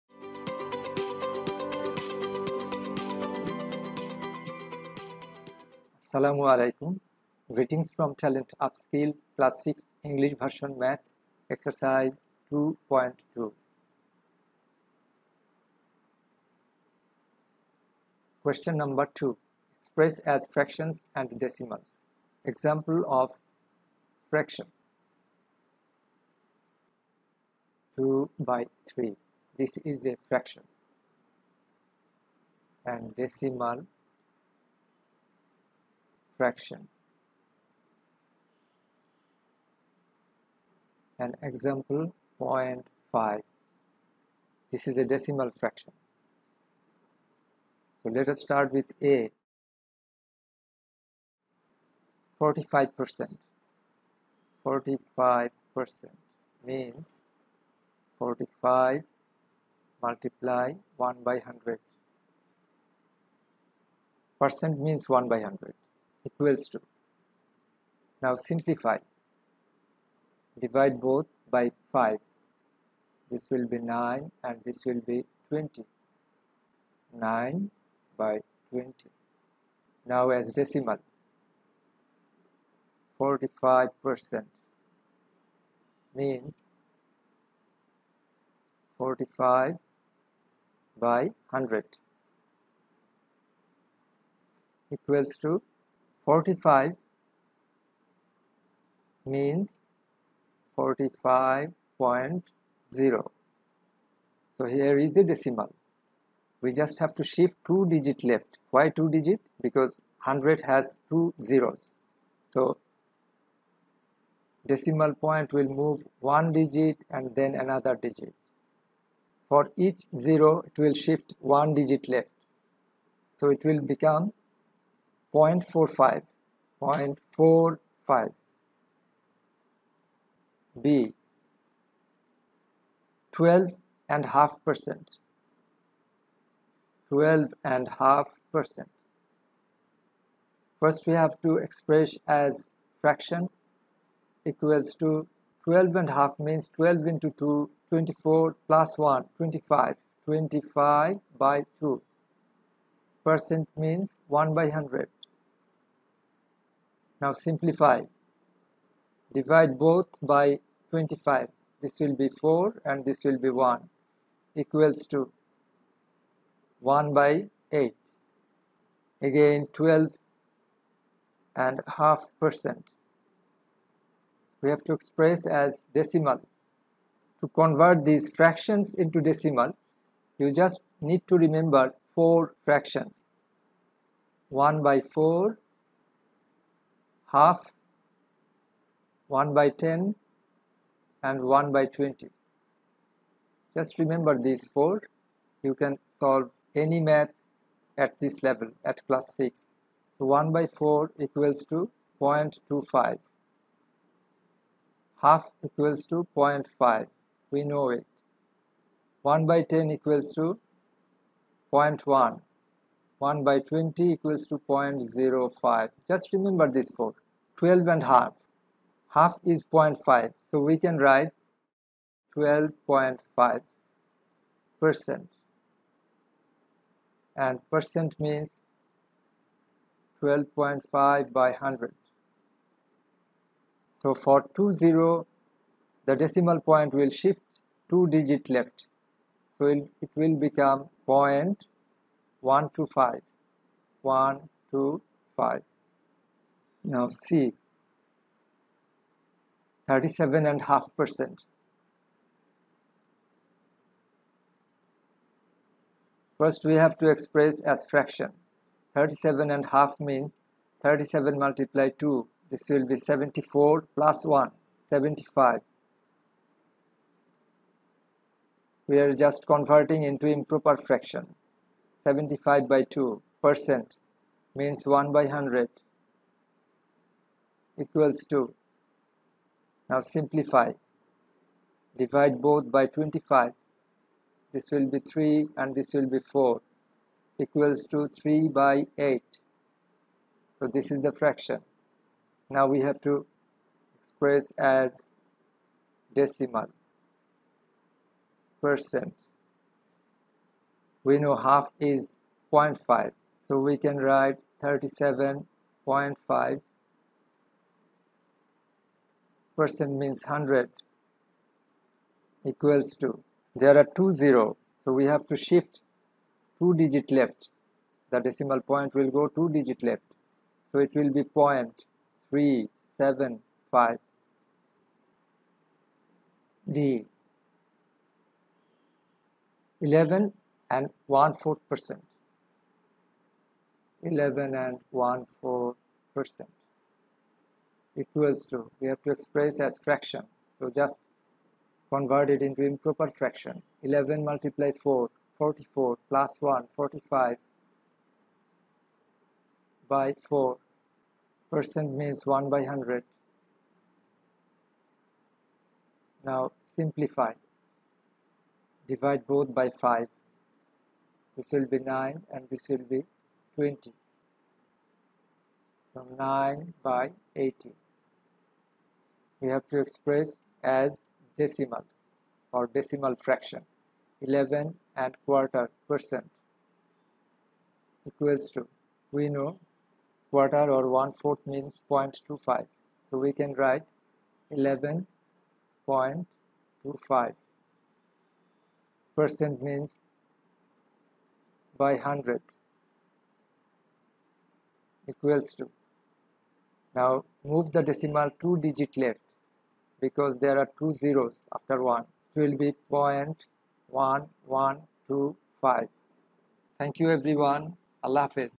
Question No 2 | Exercise 2.2 Math I Class Six English Version I NCTB I Cadet College Admission Test
Cadet College Admission Test  preparation  and English Version Class Six Math  tutorials, coaching. Free education in Bangladesh by TalentUpskill